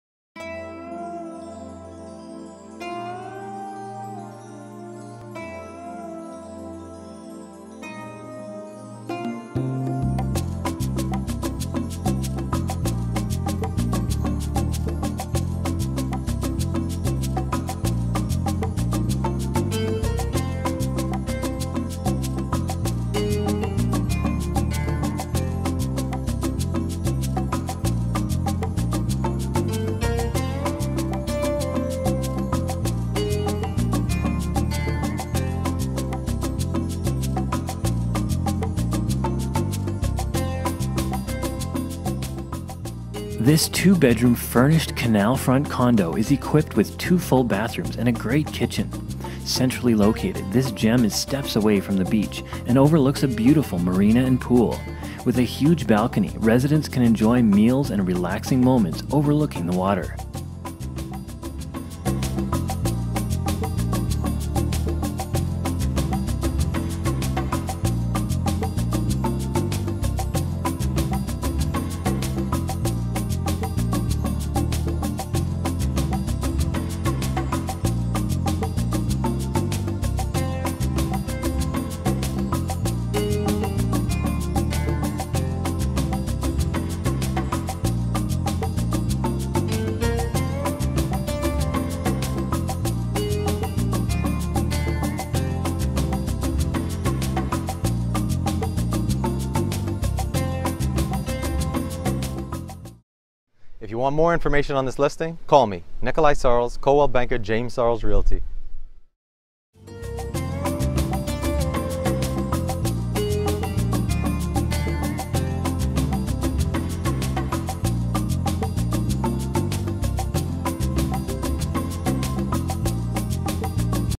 Bahamas Property - Blue Marina #401
This two bedroom furnished canal front condo is equipped with two full bathrooms and a great kitchen. Centrally located, this gem is steps away from the beach and overlooks a beautiful marina and pool. With a huge balcony, residents can enjoy meals and relaxing moments overlooking the water.